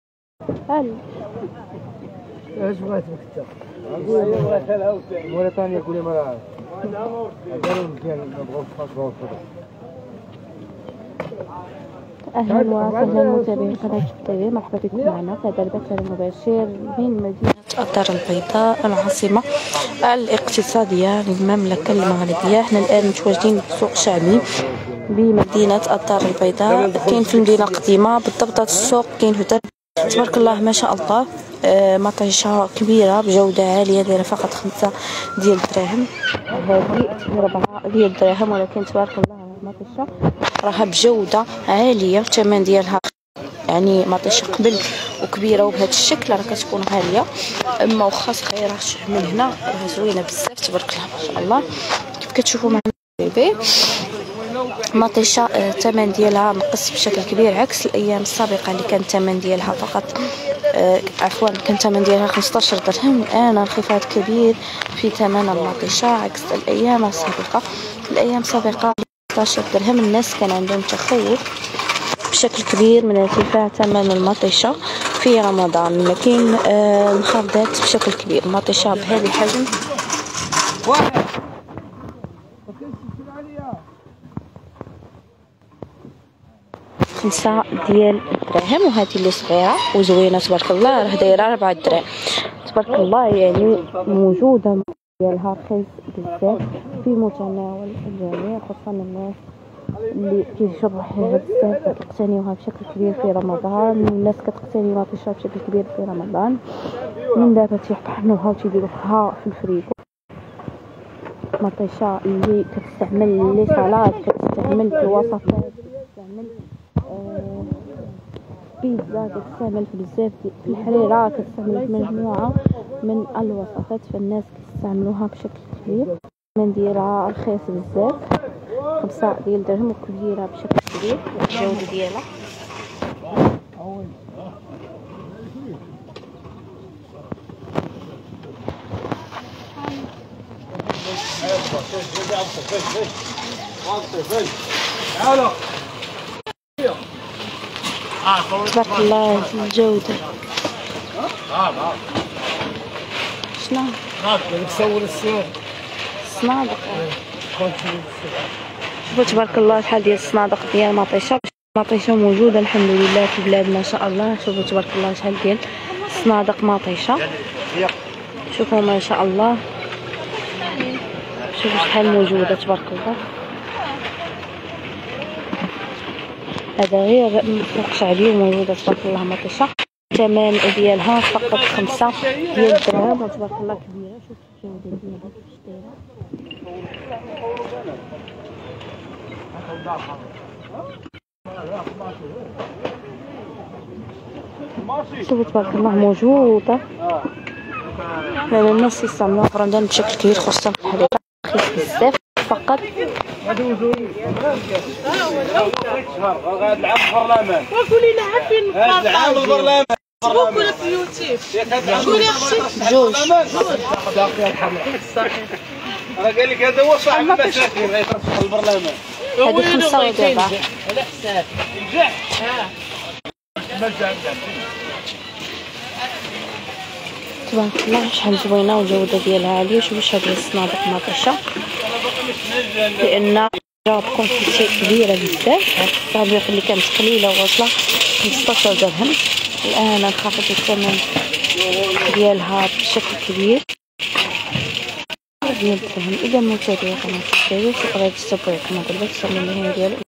مع اقتراب شهر رمضان ..مطيشة بجودة عالية ورخيصة بالأسواق الشعبية والثمن 5 دراهم للكيلو
مباشرة من الدار البيضاء..مع اقتراب شهر رمضان ..مطيشة بجودة عالية ورخيصة بالأسواق الشعبية والثمن 5 دراهم للكيلو